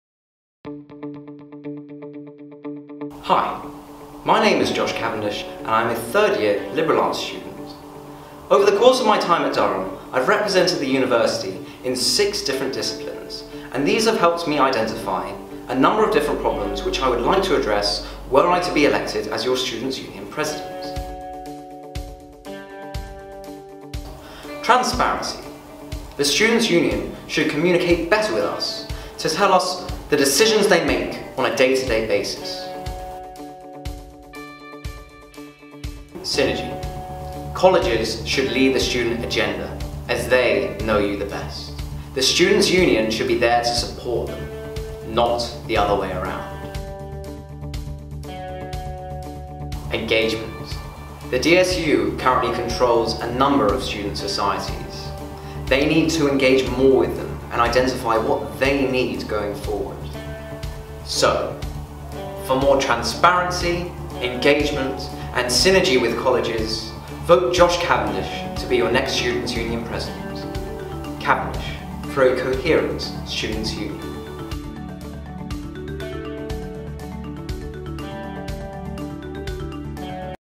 Josh Cavendish - Candidate for President
The DSU is not working. When did you last have a meaningful interaction with them? My name is Joshua Cavendish and I would like your faith in restoring it!